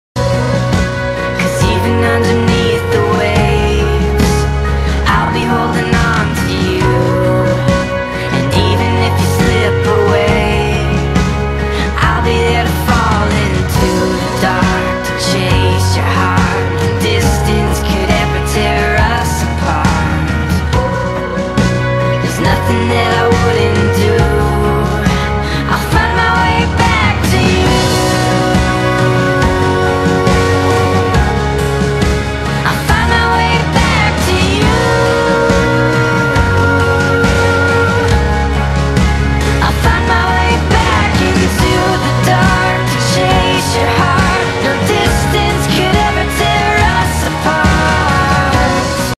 outro for xSweetStrawberryLovex
Here is your outro. I really love how it turned out and I hope you do too! Also please feel free to change the song.